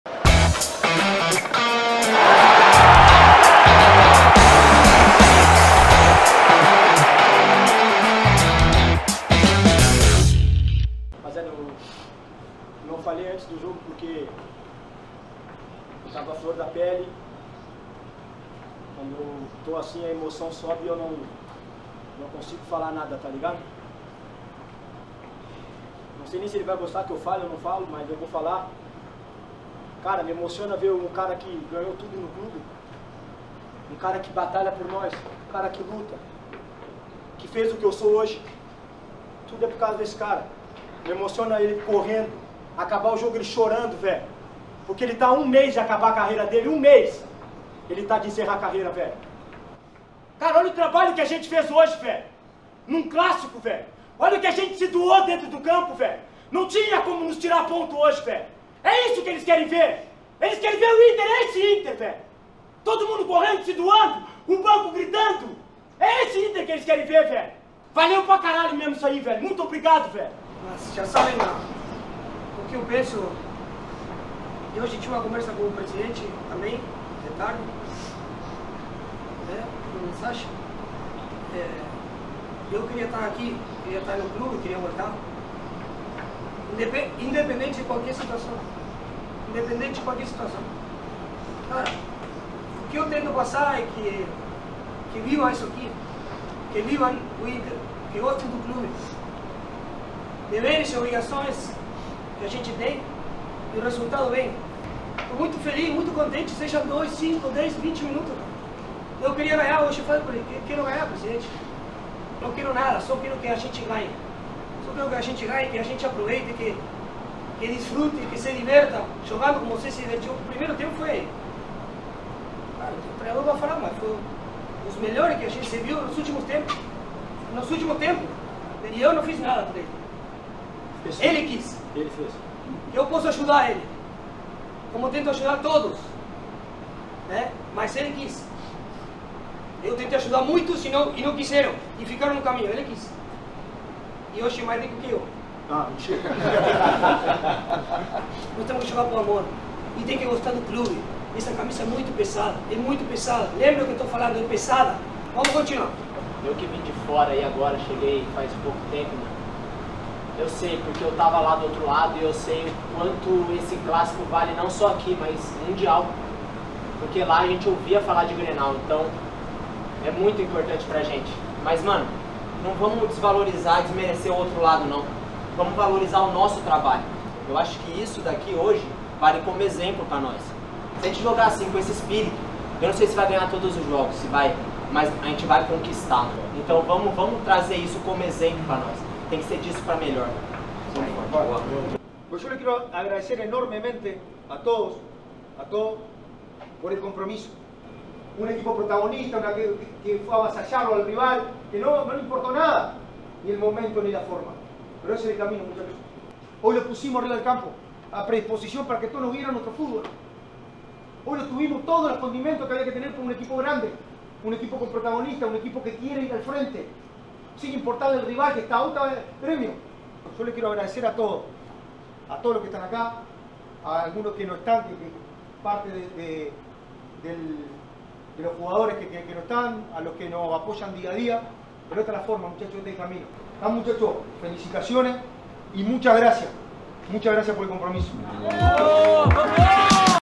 TEM QUE RESPEITAR! OS FORTES DISCURSOS NO VESTIÁRIO APÓS VITÓRIA NO GRENAL
O vestiário do Inter foi tomado por fortes e emocionantes discursos após a vitória no GreNal da última quarta-feira.

VÍDEO: TV INTER